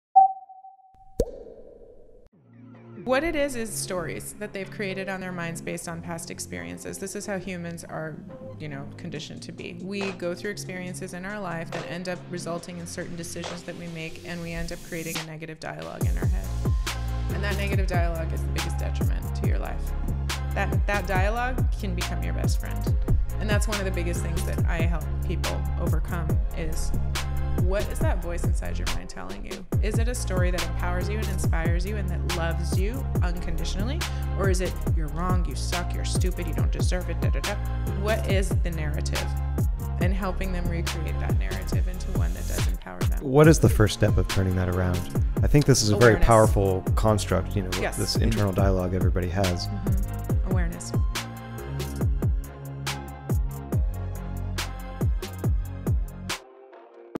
Most Important Key To Success: Self-Awareness
Cam Kashani, Godmother of Silicon Beach and Transformational Mindset Coach, speaks on the powerful truth of being truthful to yourself. Self-Awareness is the one of the most important tools an entrepreneur can have in their journey. It is rooted in every decision we make, every feeling we have and ultimately can make or break you.

CTAP Crew
Ben